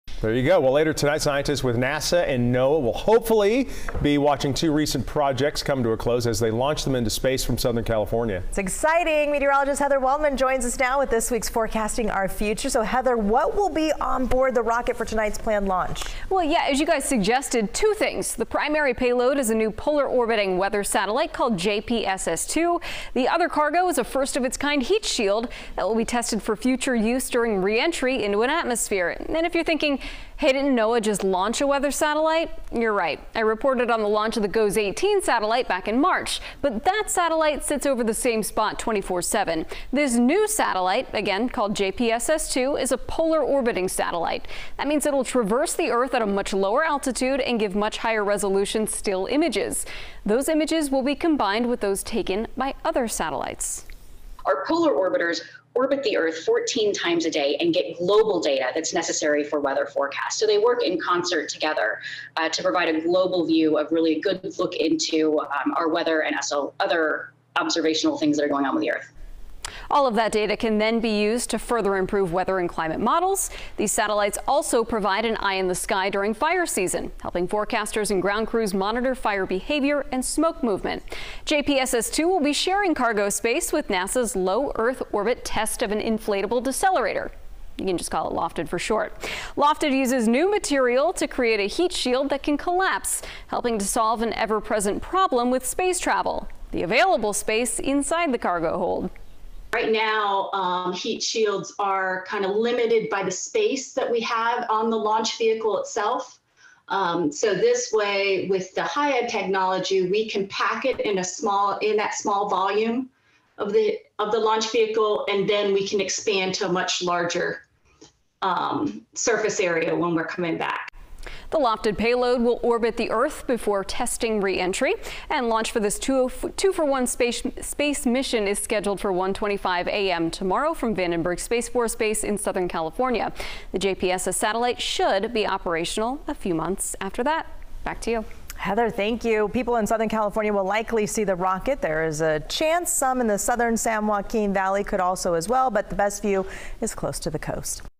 Weather satellite set to be launched overnight from Vandenberg as part of dual payload
Early Thursday morning, a rocket carrying two payloads will launch from Vandenberg Space Force Base in Southern California.  The primary payload is a new weather satellite which will be operated by the National Oceanic and Atmospheric Administration. That satellite, called JPSS-2, will orbit the Earth 14 times each day at an altitude of about 500 miles. From that relatively low height, the satellite's instruments will be able to capture very high-resolution images of events happening in the lowest part of the atmosphere.